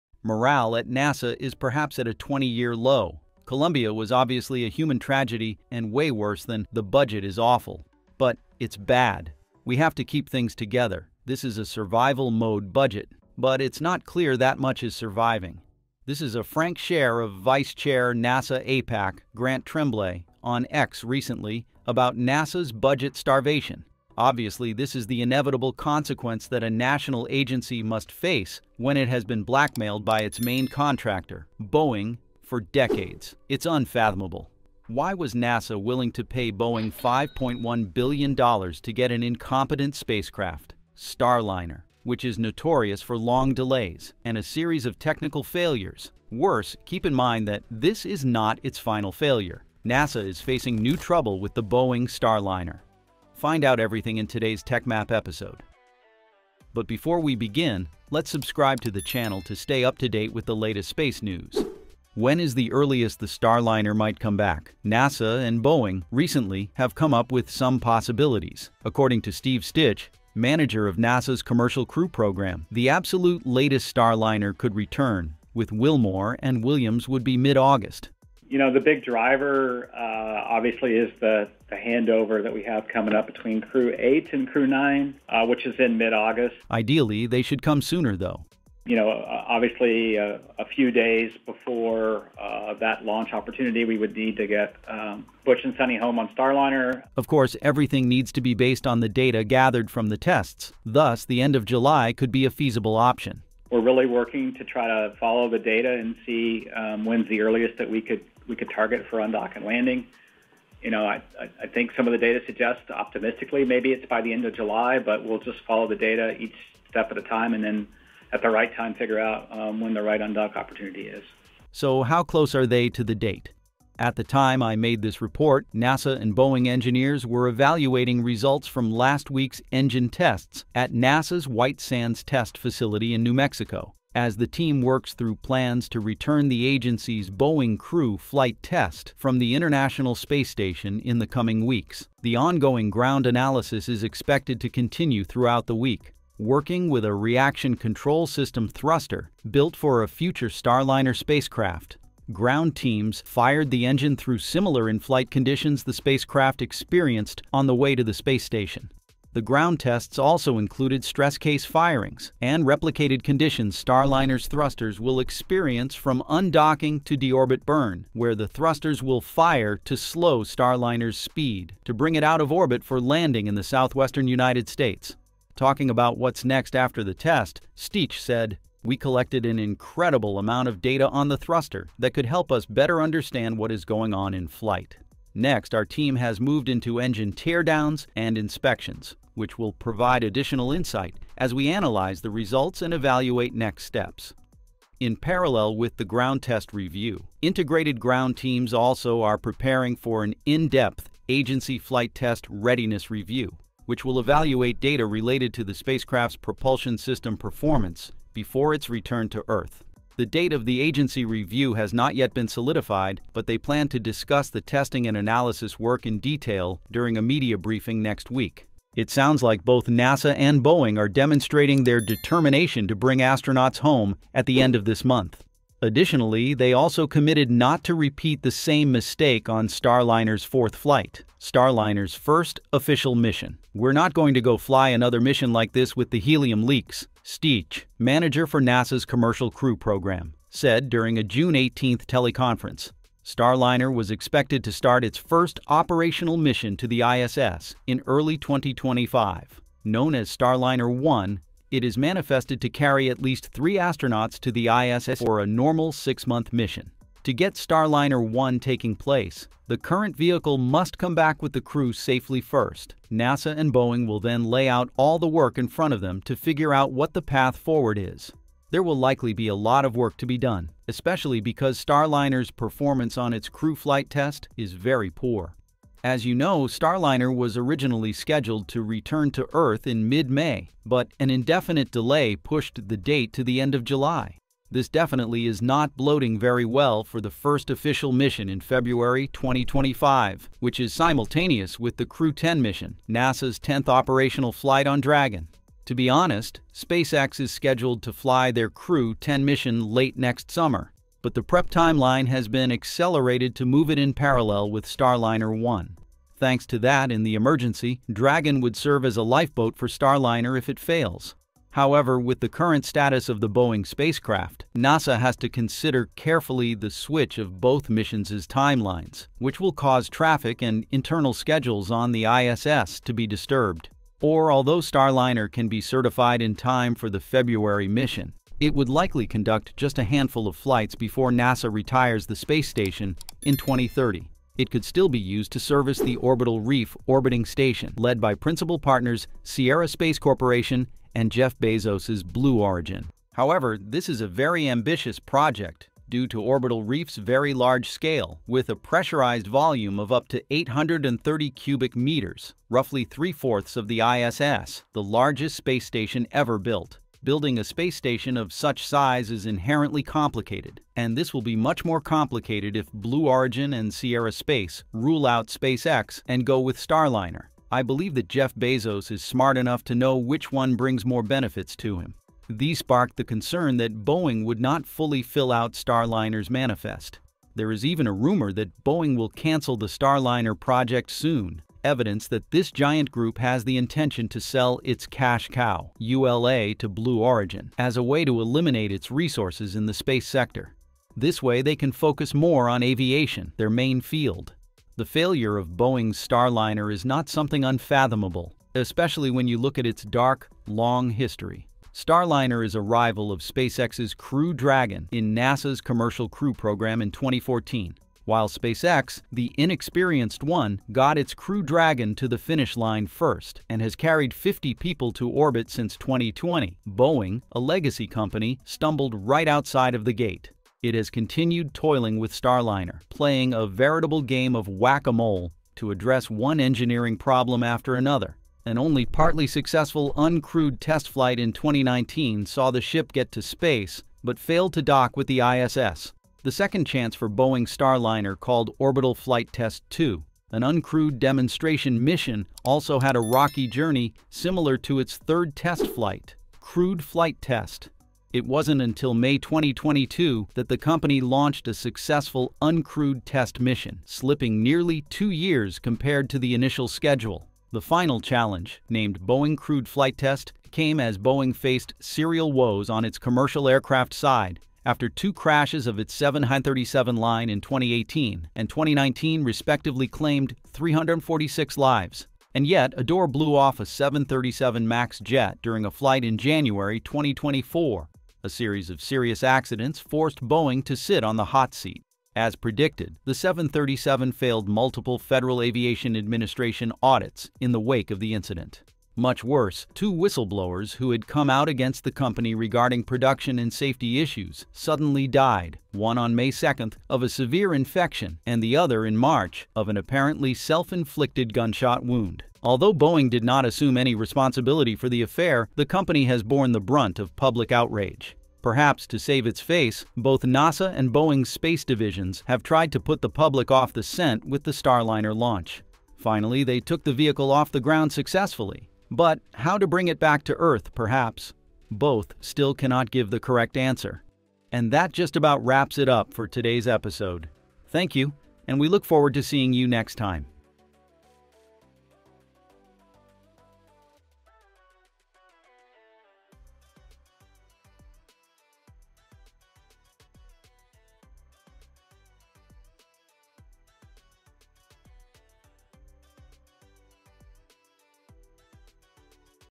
Boeing Starliner Has New Problem! NASA Confirmed "No More Future Flight''.
intro 0:00
Boeing Starliner’s update 1:02
New troubles 3:38
 A new ship with a long dark history 6:34
outro 8:44
"MORALE at Nasa is perhaps at a 20-year low! (Columbia was obviously a human tragedy and way worse than “the budget is awful”). But it’s BAD! “We have to keep things together, this is a survival-mode budget”, but it’s not clear that much is surviving."
This is a frank share of Vice-Chair Nasa Apac, Grant Tremblay, on X recently about Nasa's BUDGET STARVATION.
Obviously, this is the inevitable consequence that a national agency must FACE when it has been BLACKMAILED by its main contractor, BOEING, for decades.
It's UNFATHOMABLE! Why was Nasa willing to pay Boeing 5.1 BILLION DOLLARS to get an incompetent spacecraft, STARLINER, which is notorious for long delays, and a series of technical failures.
Worse, keep in mind that, this is NOT its FINAL FAILURE.
NASA is facing NEW TROUBLE with the BOEING STARLINER!
Find out everything in today's Techmap episode.
When is the earliest the Starliner might come back?
Nasa and Boeing, recently, have come up with some possibilities.
According to Steve Stich, manager of Nasa's Commercial Crew Program, the absolute latest Starliner could return with Wilmore and Williams would be mid-August.
 "The big driver is the handover that we have coming up between Crew-8 and Crew-9, which is in mid-August,"
Ideally, they should come sooner, though.
" you know obviously a few days before uh that launch opportunity we would need to get um Bush and sunny home on Starliner"
Of course, everything needs to be based on the data gathered from the tests, thus, the end of July could be a feasible option.
"We're really working to try to follow the data and see when's the earliest that we could target for undock and landing. I think some of the data suggests, optimistically, maybe it's by the end of July, but we'll just follow the data each step at a time, and figure out when the right undock opportunity is."
So how close are they to the date?
At the time I made this report, Nasa and Boeing engineers were evaluating results from last week’s engine tests at Nasa’s White Sands Test Facility in New Mexico as the team works through plans to return the agency’s Boeing Crew Flight Test from the International Space Station in the coming weeks.
The ongoing ground analysis is expected to continue throughout the week.
Working with a reaction control system thruster built for a future Starliner spacecraft, ground teams fired the engine through similar inflight conditions the spacecraft experienced on the way to the space station. The ground tests also included stress-case firings, and replicated conditions Starliner’s thrusters will experience from undocking to deorbit burn, where the thrusters will fire to slow Starliner’s speed to bring it out of orbit for landing in the southwestern United States.